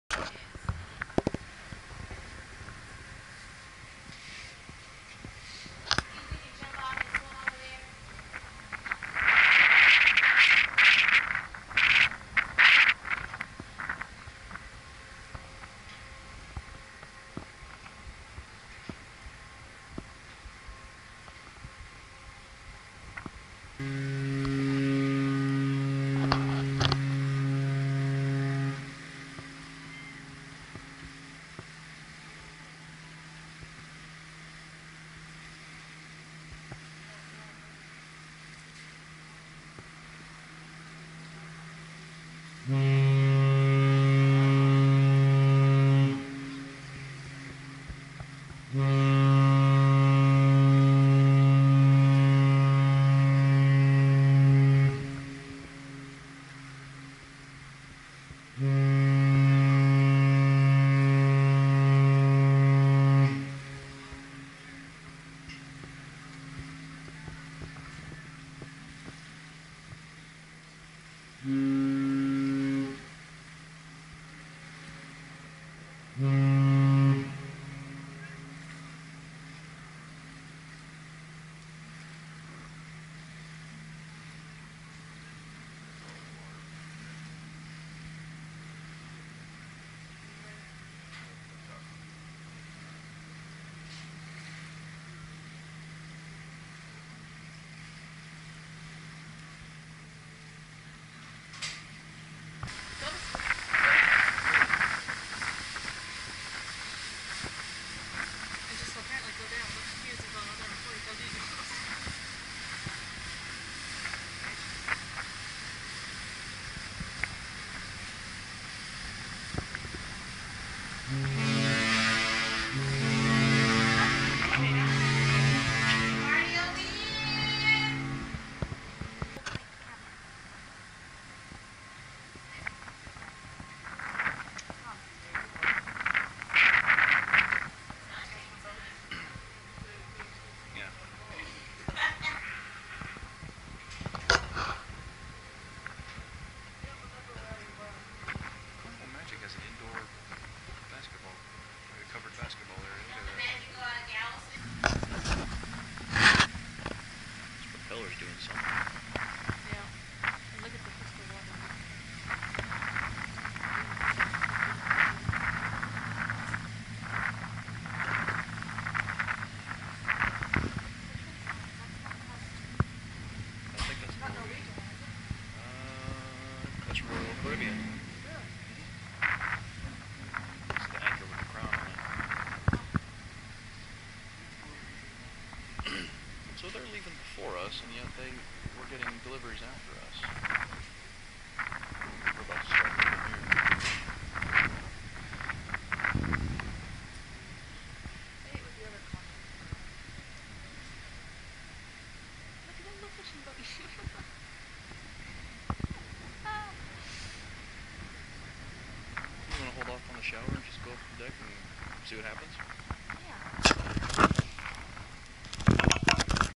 Cruise Ship Horn Duel At Grand Cayman
Here are a few clips recorded from my balcony on the Carnival Sunshine while we were about to leave Grand Cayman.  The ships were having a horn duel.

Filmed on my Sony Action Cam HDR-AS30V.